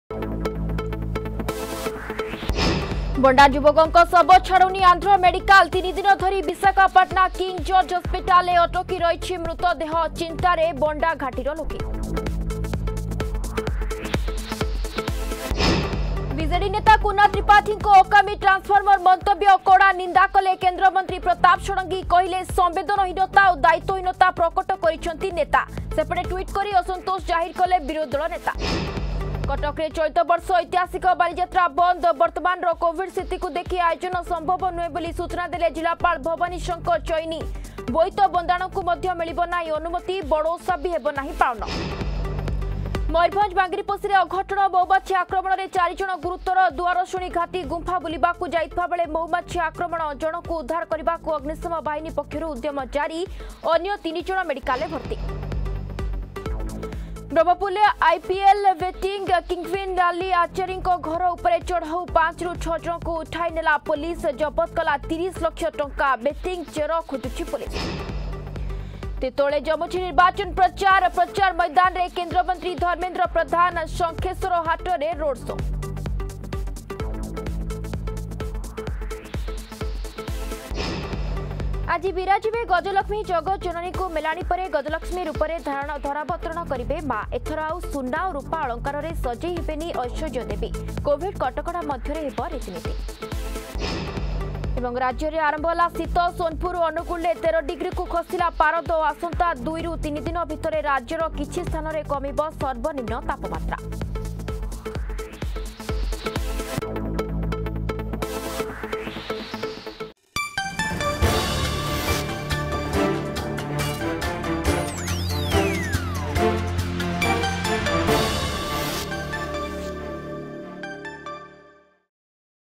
Headlines 12Pm
ବଣ୍ଡା ଯୁବକଙ୍କ ଶବ ଛାଡୁନି ଆନ୍ଧ୍ର ମେଡିକାଲକଟକରେ ଚଳିତ ବର୍ଷ ଐତିହାସିକ ବାଲିଯାତ୍ରା ବନ୍ଦ...ମୟୁରଭଞ୍ଜ ବାଙ୍ଗିରିପୋଷିରେ ଅଘଟଣବ୍ରହ୍ମପୁରରେ ଆଇପିଏଲ ବେଟିଂ. ପ୍ରଚାର ମୈଦାନରେ କେନ୍ଦ୍ରମନ୍ତ୍ରୀ ଧର୍ମେନ୍ଦ୍ର ପ୍ରଧାନ...